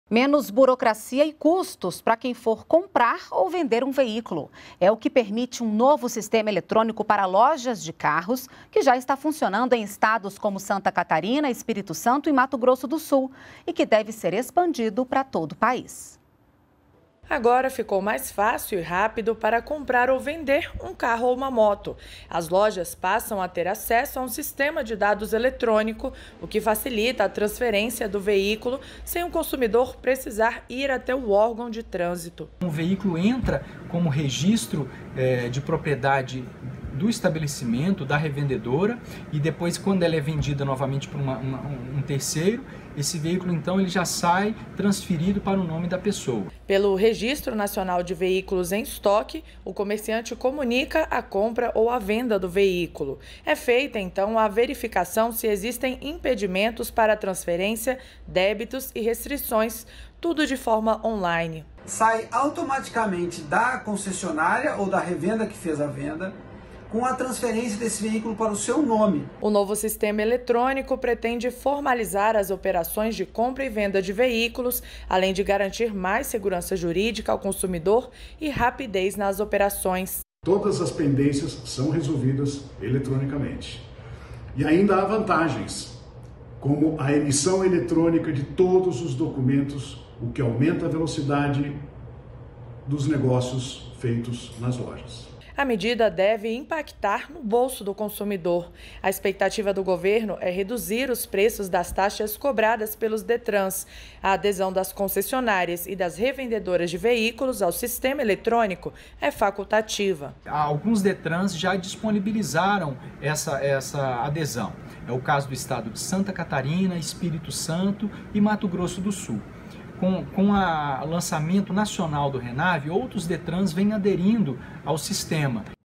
Novo sistema permite transferência eletrônica de propriedade de carros e motos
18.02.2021 - Menos burocracia e custos para quem for comprar ou vender um veículo. É o que permite um novo sistema eletrônico para as lojas de carros, que já está funcionando em estados como Santa Catarina, Espírito Santo e Mato Grosso do Sul, e que deve ser expandido para todo o país.